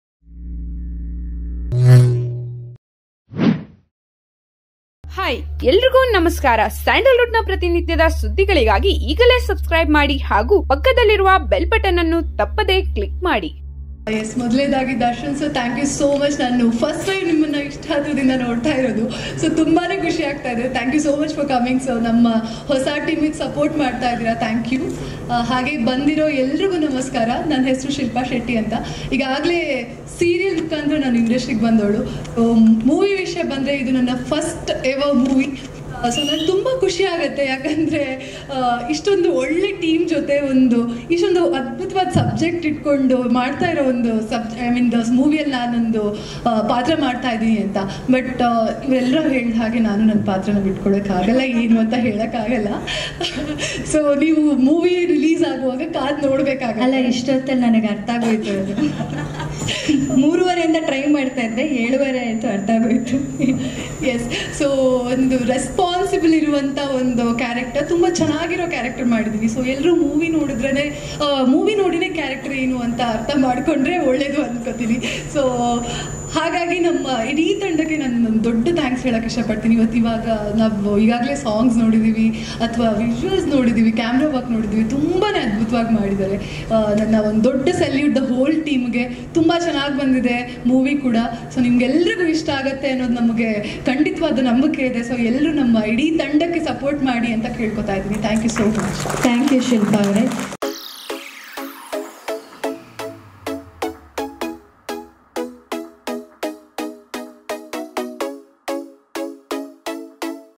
ದರ್ಶನ್ ಸರ್ ಅವರನ್ನ ಇದೆ ಮೊದಲು ಇಷ್ಟು ಹತ್ತಿರದಿಂದ ನೋಡಿತ್ತಿರೋದು ತುಂಬಾ ಖುಷಿ ಆಗ್ತಿದೆ | Shilpa Shetty
Kannada Actress Shilpa Shetty Speaks About D Boss Darshan At Neuron Kannada Movie Audio Launch Trailer Songs Teaser Release Function Challenging Star Darshan Latest Updates